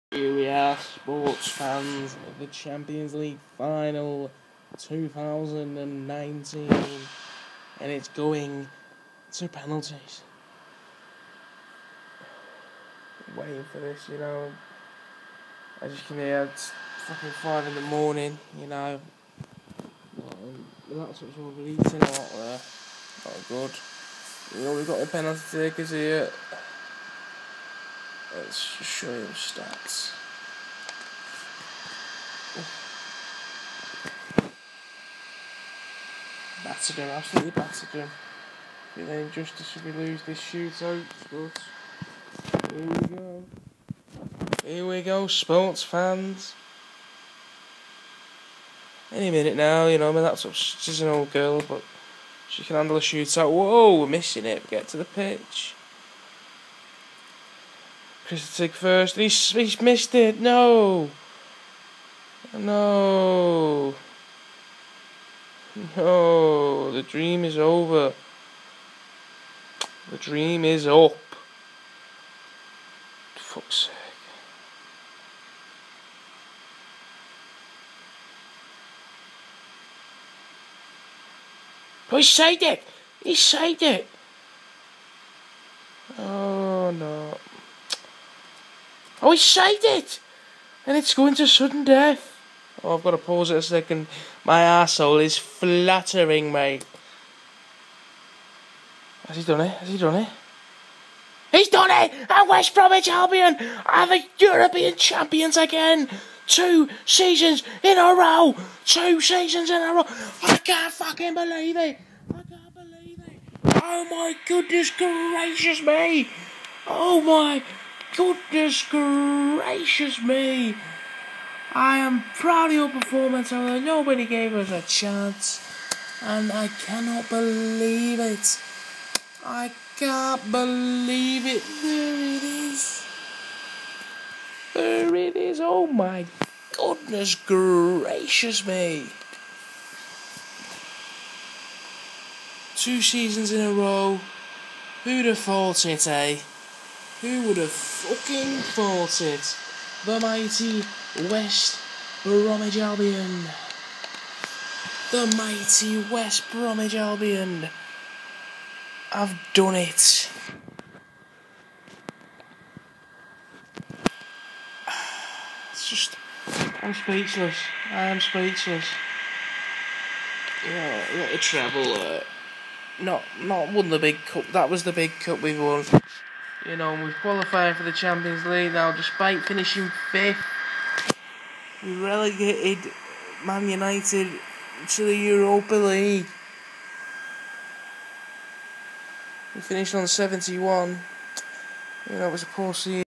2019 UEFA Champions League Final Penalty Shootout
West Brom were looking to win their second consecutive champions league and after 120 gruelling minutes vs Man Utd the score was deadlocked at 1-1. Who snatched it on penalties??.